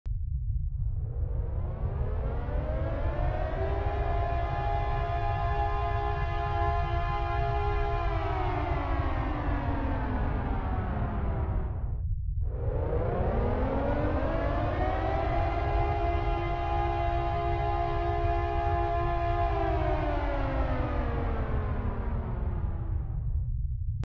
BLACK HOLE IN MINECRAFT ABSORBS THE ATMOSPHERE
If a black hole formed next to the world in Minecraft...
CGI arts with Minecraft objects.